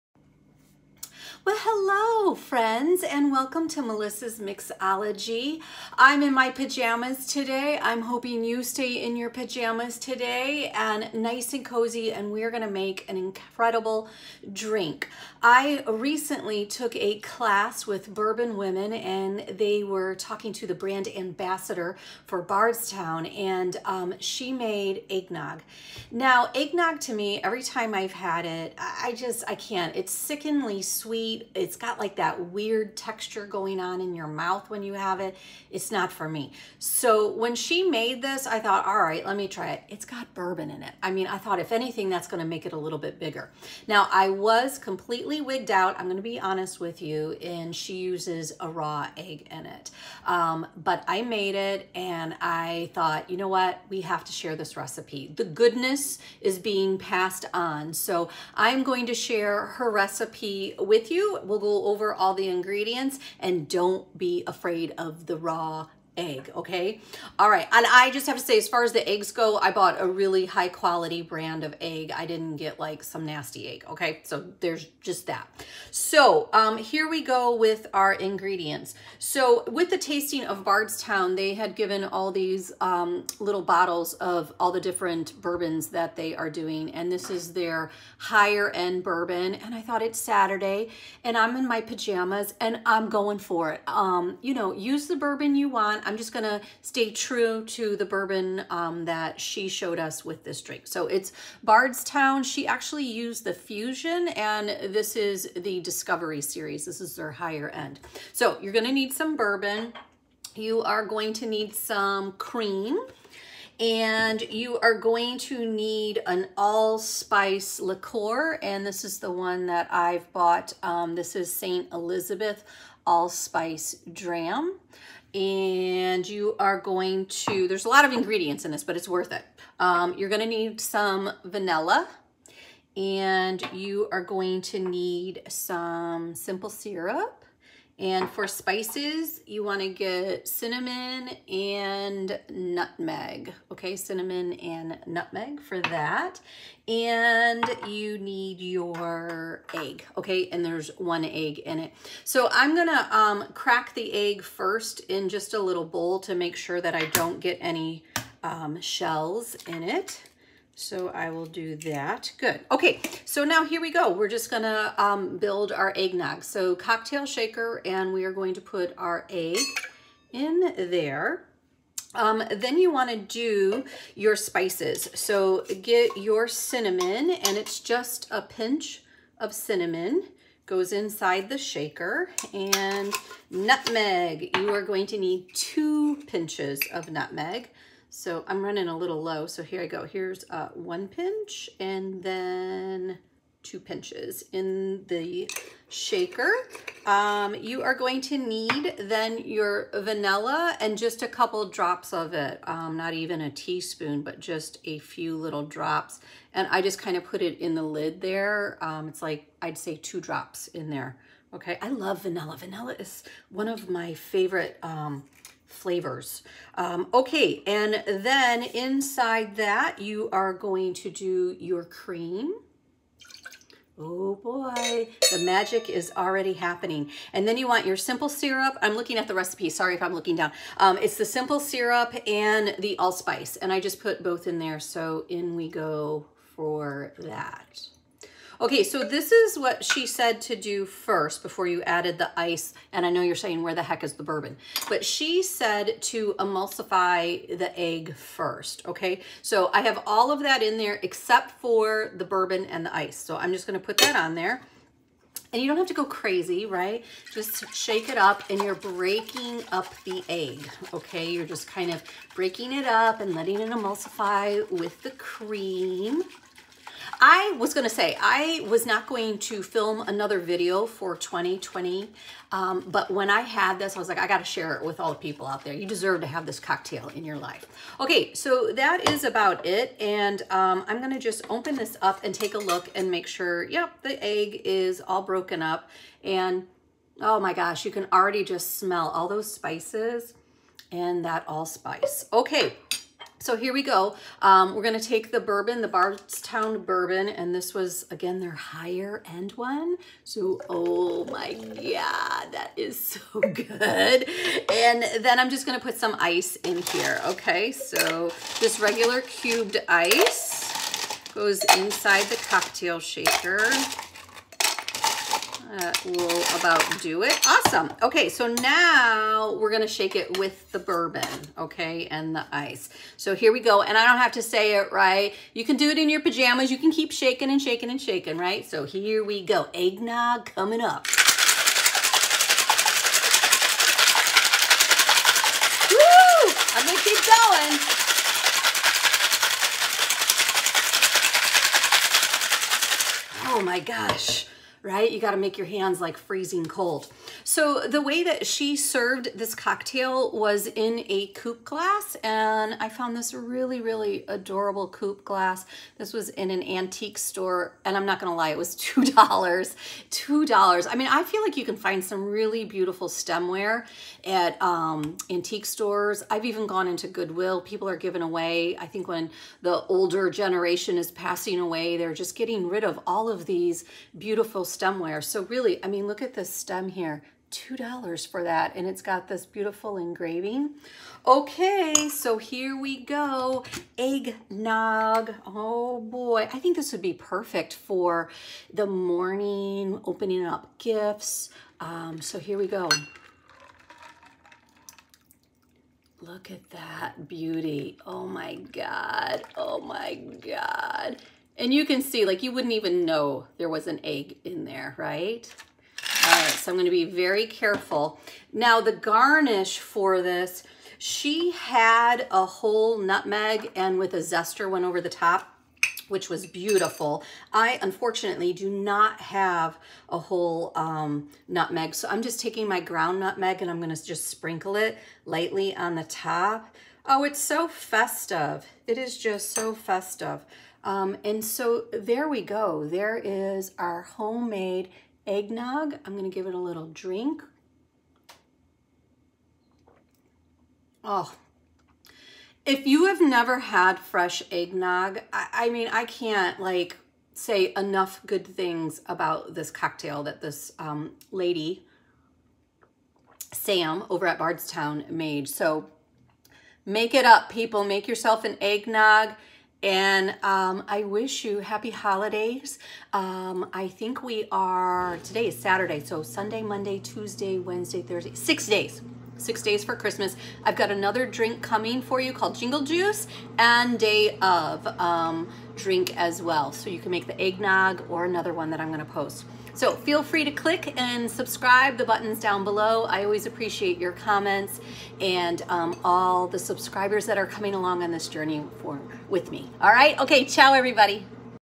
Holiday Eggnog
1.5 oz bourbon
1 oz simple syrup
1/4 oz allspice liquor
3/4 oz cream
1 fresh egg
2 drops vanilla
2 pinches nutmeg
1 pinch cinnamon

To cocktail shaker add all ingredients but bourbon and ice.  Shake to emulsify the egg.

Then add bourbon and ice.  Shake baby shake.

Pour into a coupe glass- garnish with nutmeg shavings. Enjoy!